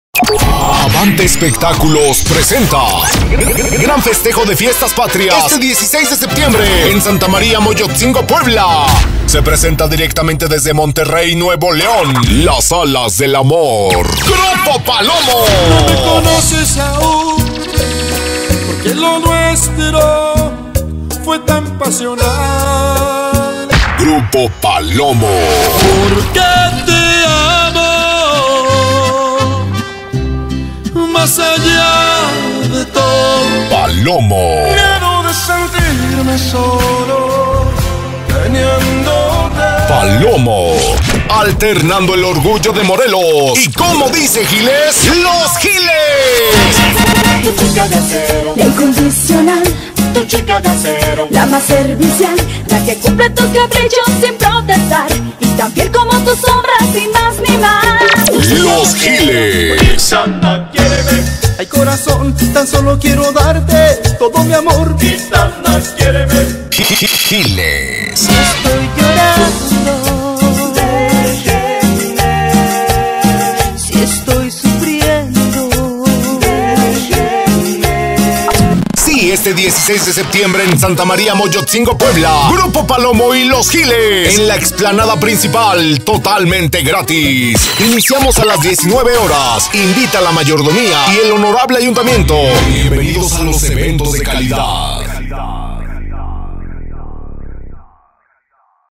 PROMO MOYOTZINGO PUEBLA / PALOMO Y LOS GILES / 16 SEPTIEMBRE FIESTAS PATRIAS
Gran Bailazo de Fiestas Patrias en Santa_Maria_Moyotzingo Puebla con la presencia desde Monterrey Palomo y el orgullo de Morelos Los Giles. Este 16 de Septiembre, No te lo puedes perder!!!